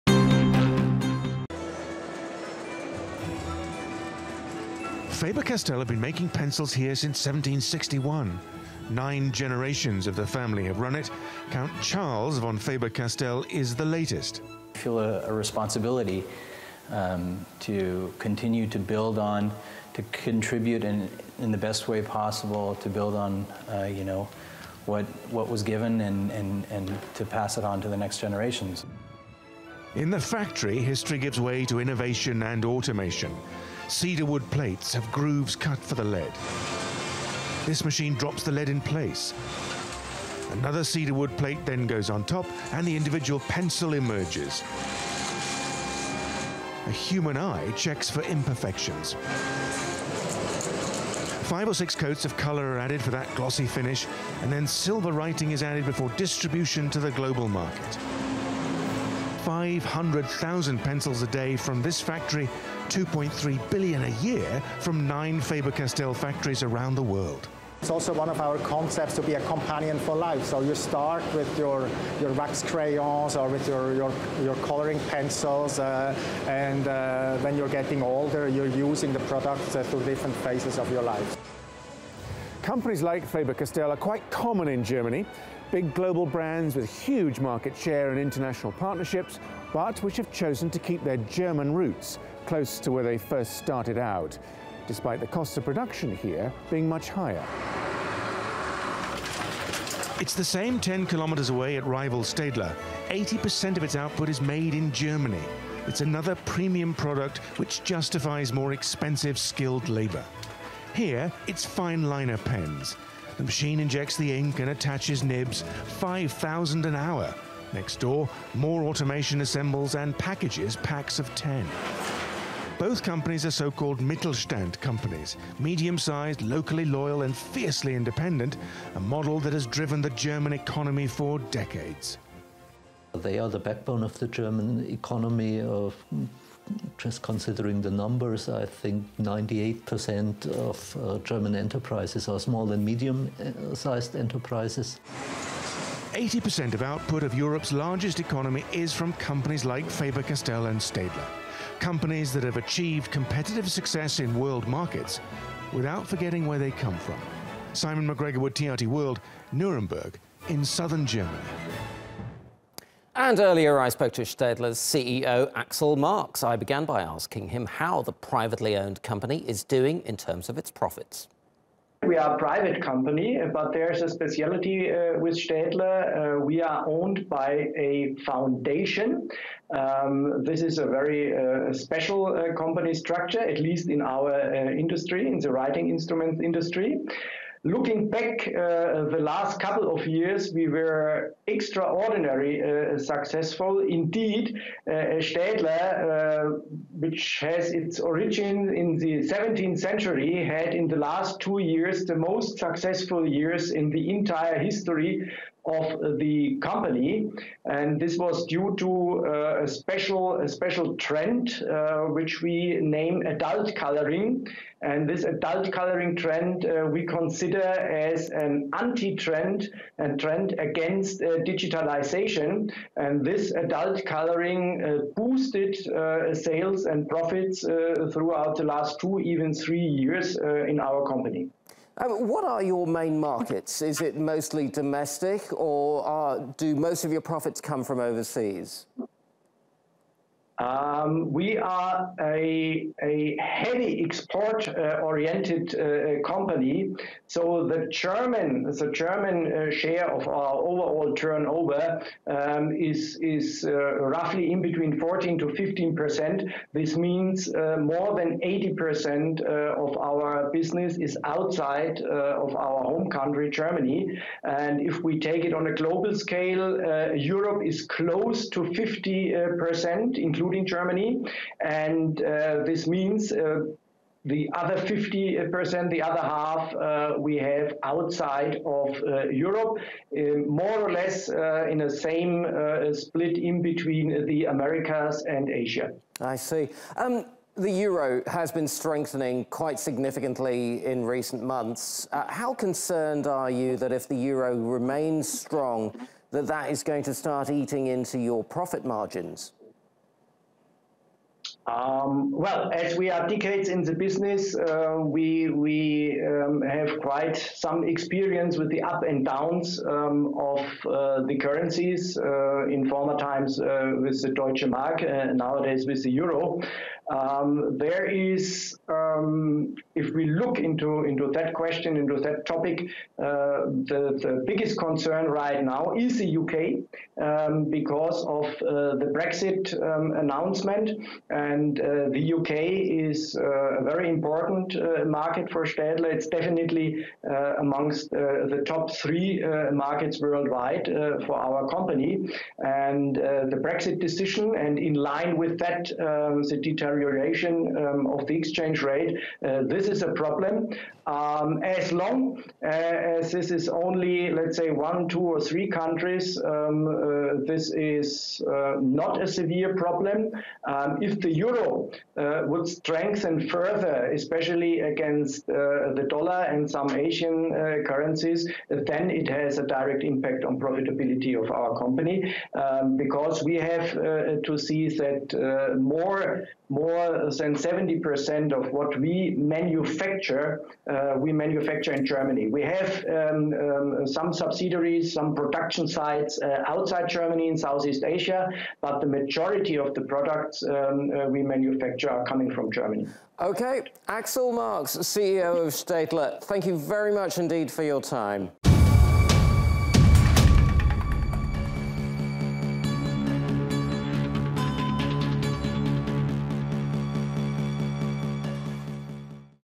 Money Talks: Nuremberg pencil makers drive German economy
The German city of Nuremberg has been the centre of pencil making for centuries. Two of the world's leading brands, Faber Castell and Staedler, are based within 10 km of each other. They are examples of what is called the Mittelstand, successful medium sized companies which are the backbone of Germany's thriving economy. Simon McGregor-Wood went to take a look. We also speak to Staedtler CEO, Axel Marx.